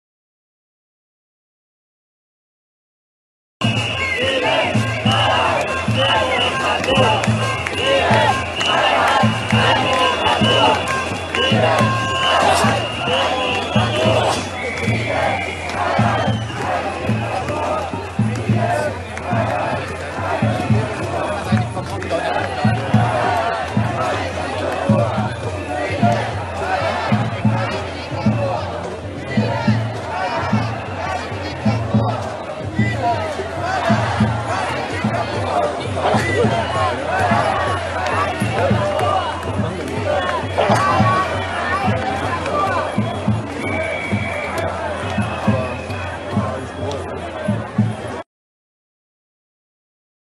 German protest against lockdown and restrictions 1st August 2021
Large protest in Germany against lockdown and restrictions 1st August 2021